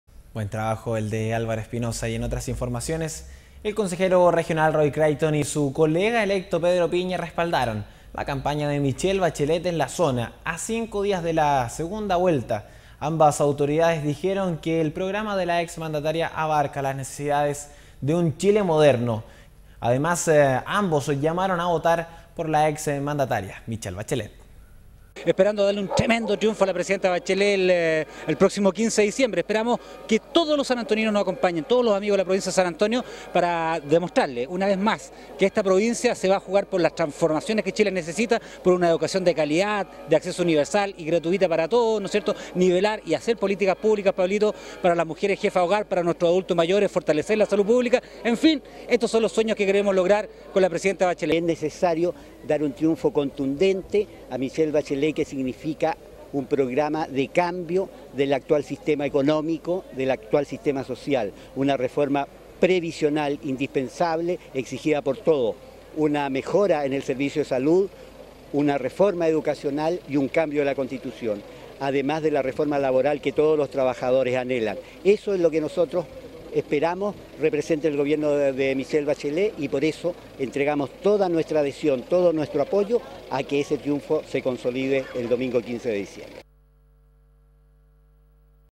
10 DIC 2013 APOYAN A MICHELE BACHELET
EL CONSEJERO REGIONAL ROY CRICHTON Y SU COLEGA ELECTO, PEDRO PIÑA, RESPALDARON LA CAMPAÑA DE MICHELLE BACHELET EN LA ZONA, A CINCO DÍAS DE LA ELECCIÓN DE LA SEGUNDA VUELTA.
AMBAS AUTORIDADES DIJERON QUE EL PROGRAMA DE LA EX MANDATARIA ABARCA LAS NECESIDADES QUE UN CHILE MODERNO REQUIERE. POR ELLO, AMBOS LLAMARON A VOTAR POR LA EX MANDATARIA MICHELLE BACHELET.